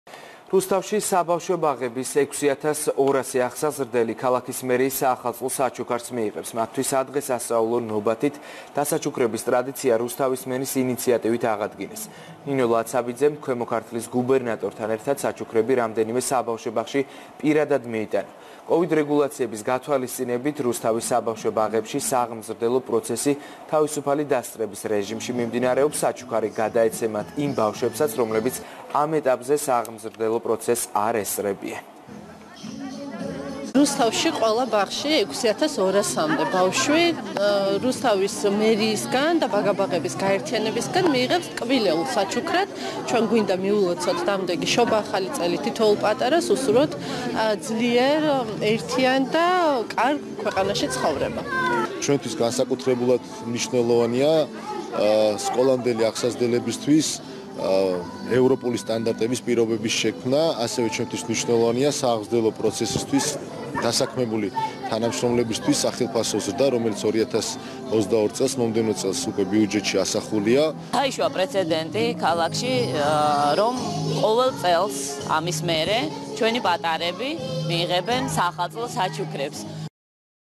სადღესასწაულო საჩუქრები რუსთავის ბაღების აღსაზრდელებისთვის
რუსთავში, საბავშვო ბაღების 6200 აღსაზრდელი ქალაქის მერიის საახალწლო საჩუქარს მიიღებს.
ბაღების აღსაზრდელების სადღესასწაულო ნობათით დასაჩუქრების ტრადიცია, რუსთავის მერის ნინო ლაცაბიძის ინიციატივით აღადგინეს.
34-ე ბაღის აღსაზრდელებს საჩუქრები ქვემო ქართლში სახელმწიფო რწმუნებულმა გიორგი დოხტურიშვილმა და რუსთავის მერმა პირადად გადაცეს.
რუსთავის საბავშვო ბაღების გაერთიანების ოცდაორივე ფილიალში, სადღესასწაულო საჩუქარი საშობაო არდადეგების დაწყებამდე დარიგდება.
კოვიდრეგულაციების გათვალისწინებით, რუსთავის საბავშვო ბაღებში, სააღმზრდელო პროცესი თავისუფალი დასწრების რეჟიმში მიმდინარეობს.
საჩუქარი გადაეცემათ იმ ბავშვებსაც, რომლებიც ამ ეტაპზე, სააღმზრდელო პროცესს არ ესწრებიან.
საშობაო არდადეგები რუსთავის საბავშვო ბაღებში 25 დეკემბრიდან 17 იანვრამდე გაგრძელდება